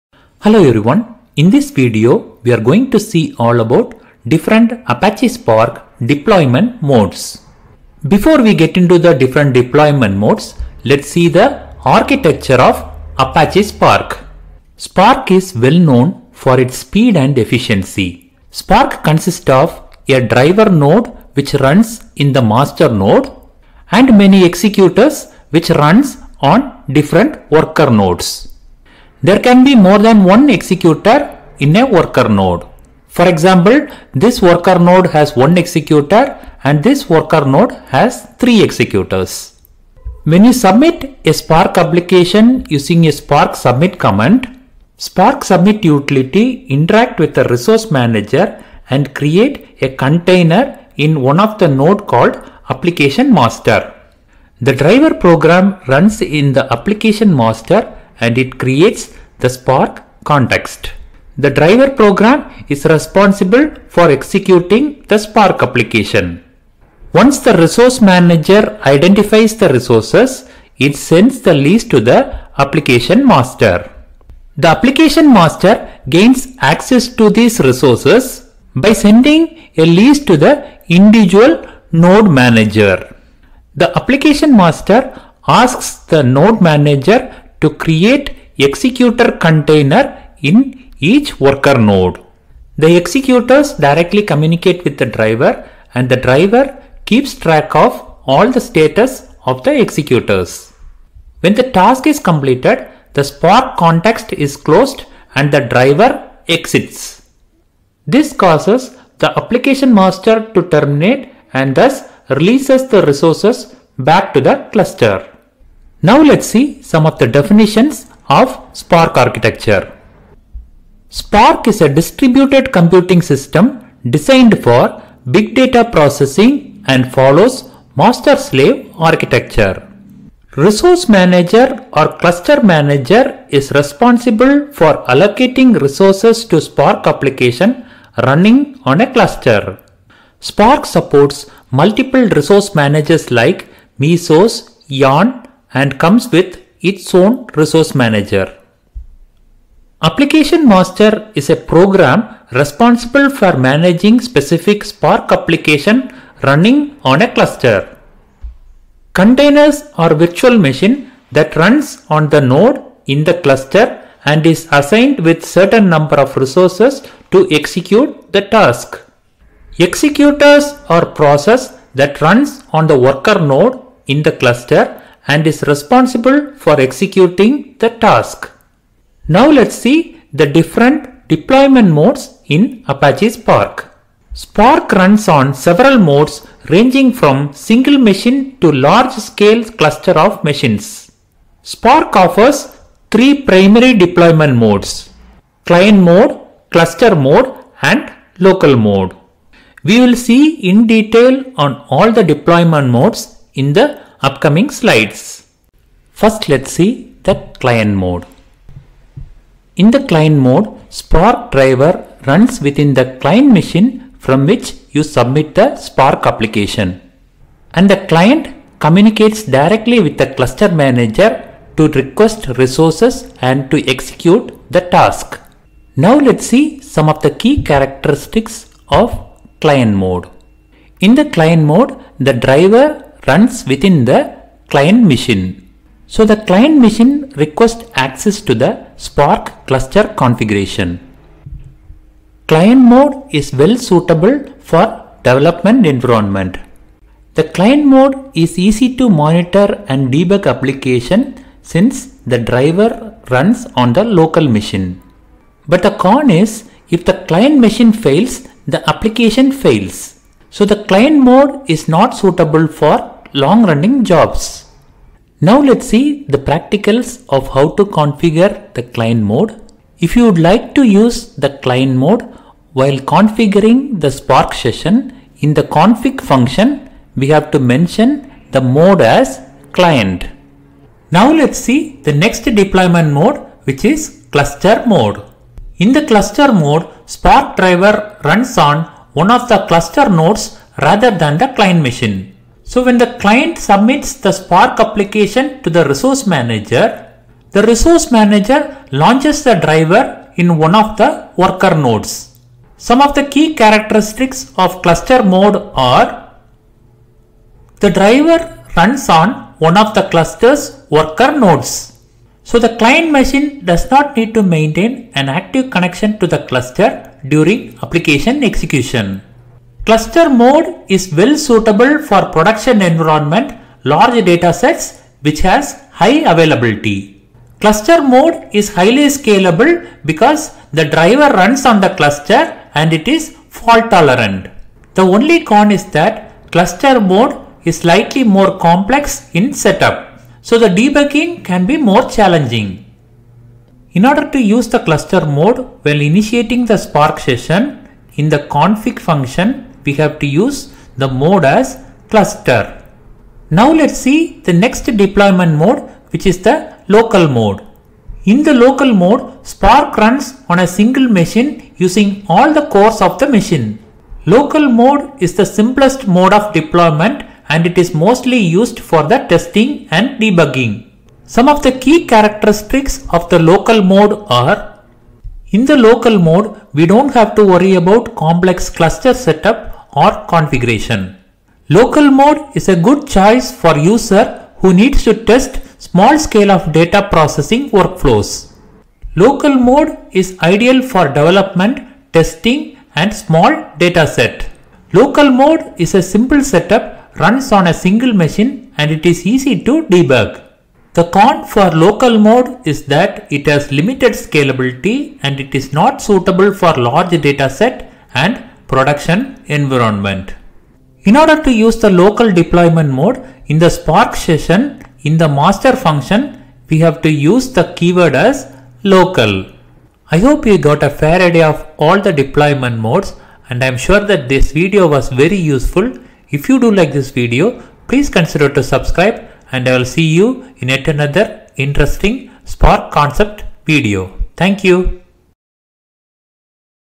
Apache Spark Deployment Modes
Spark runs in several modes, ranging from a single machine to a large-scale cluster of machines.
Offers three primary deployment modes
Client mode
Cluster mode
Local mode